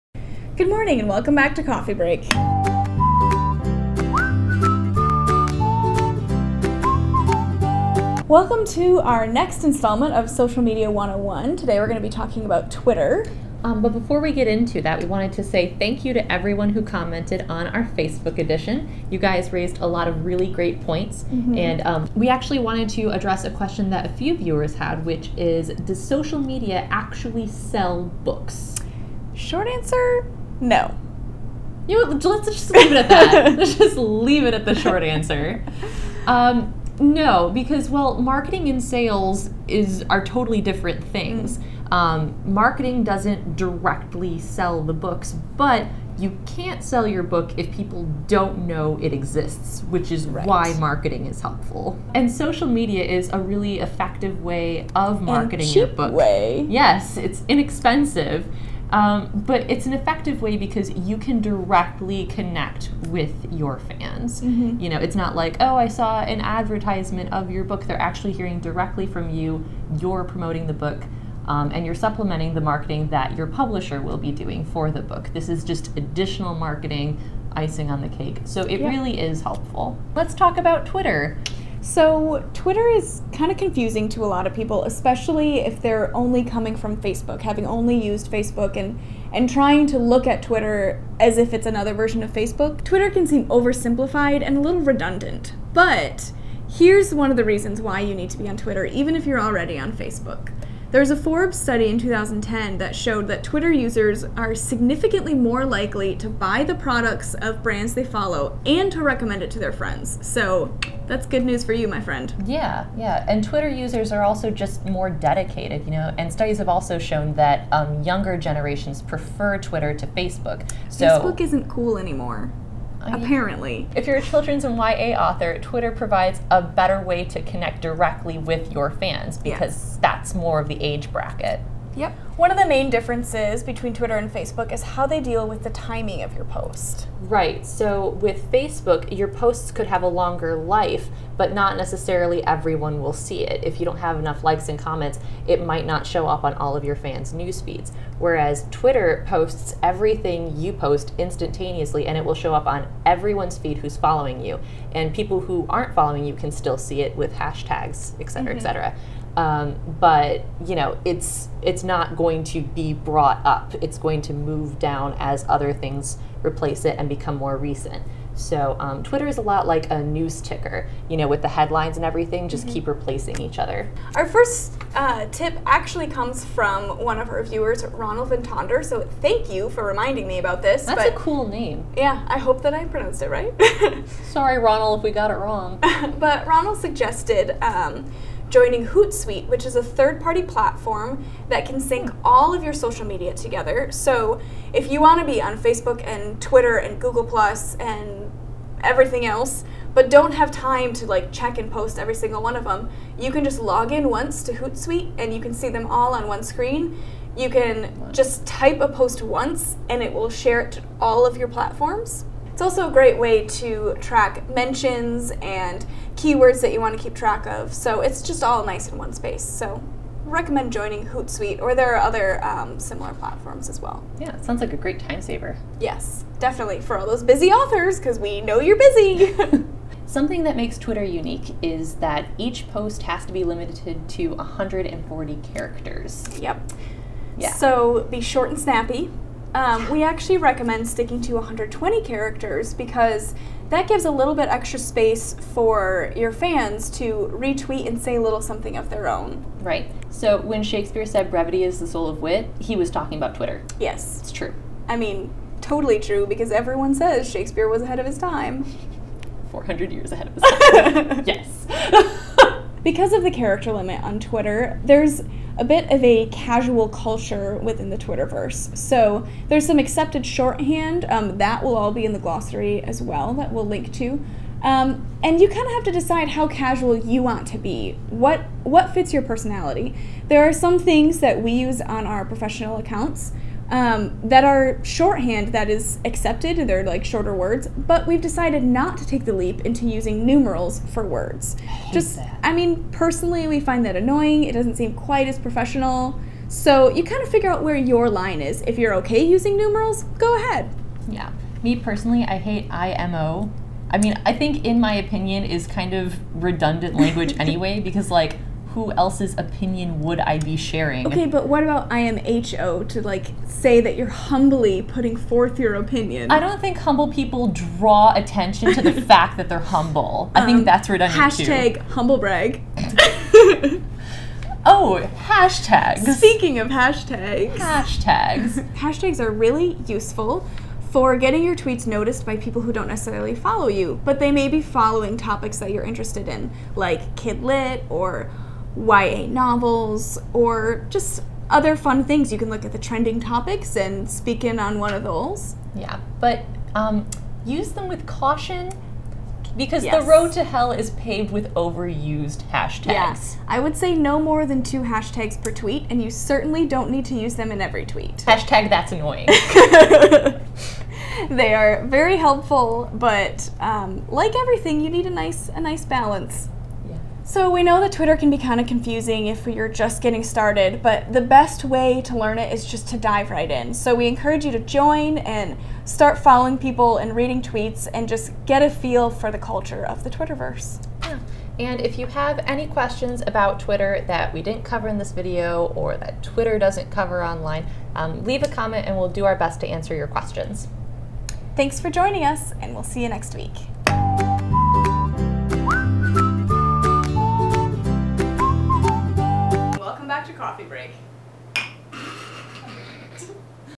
{SM101} Twitter Tips for Authors
Today Ahna and Katherine discuss Twitter, why you should have an account even if you're already on Facebook, why (or why not) to use hashtags, and more! Thanks to Ronel van Tonder for our first tip!

Resources:
Eerdmans' own social media guide for authors (this information pulled from section III)